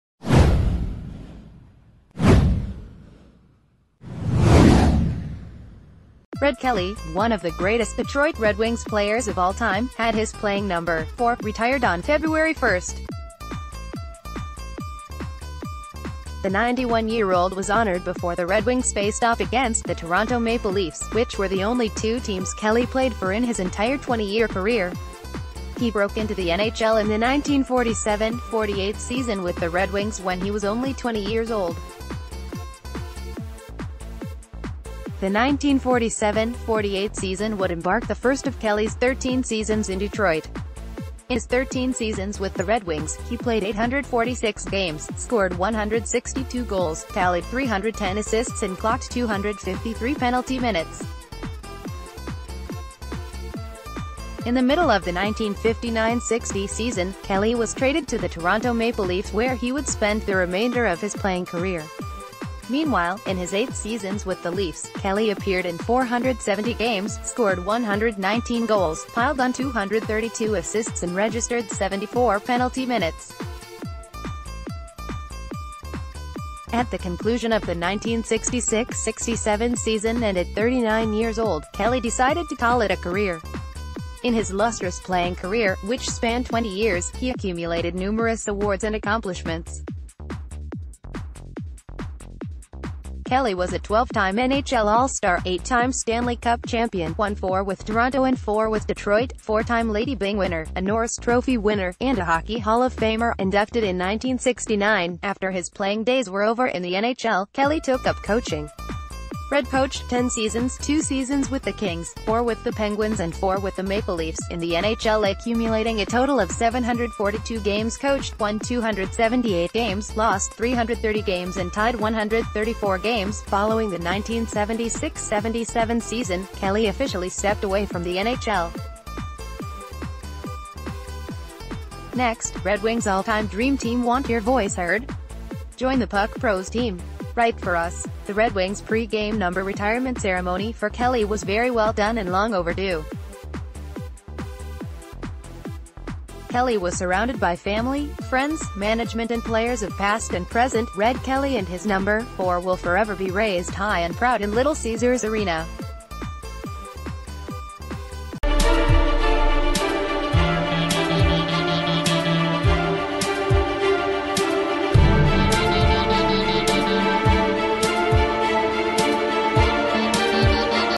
Detroit Red Wings Retire Red Kelly’s Number
Red Kelly, one of the greatest Detroit Red Wings players of all time, had his playing number, four, retired on February 1st.  The 91-year-old was honored before the Red Wings faced off against the Toronto Maple Leafs, which were the only two teams Kelly played for in his entire 20-year career. He broke into the NHL in the 1947-48 season with the Red Wings when he was only 20 years old. The 1947-48 season would embark the first of Kelly’s 13 seasons in Detroit.  In his 13 seasons with the Red Win...